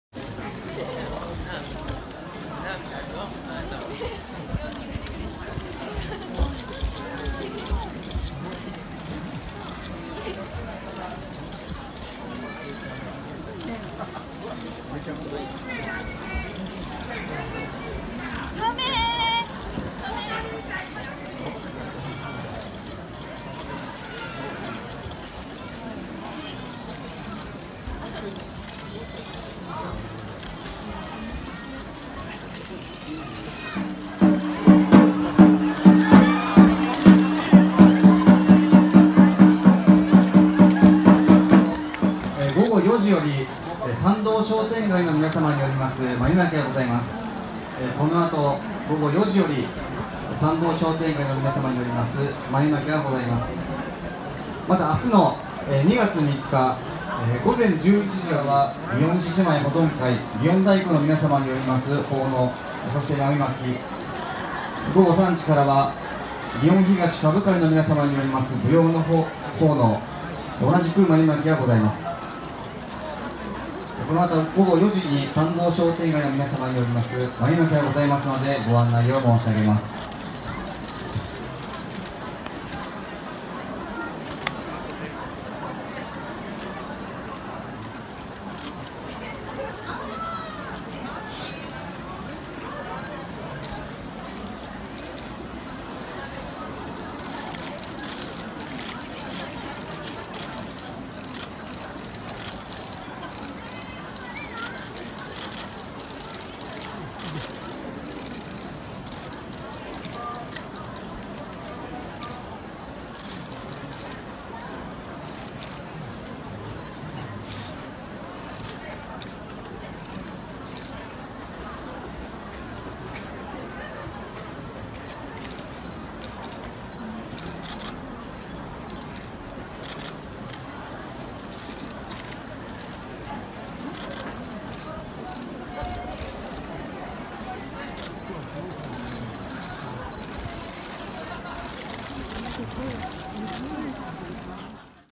宮川町舞妓（Maiko）千賀染さんの豆まき（八坂神社節分祭） 2019.2.2
八坂神社の宮川町の舞妓さんの節分祭の豆まきの様子です。宮川町の舞妓さんたちが、豆まきを行いました。八坂神社境内には、豆をもらうために大勢の人たちが集まりました。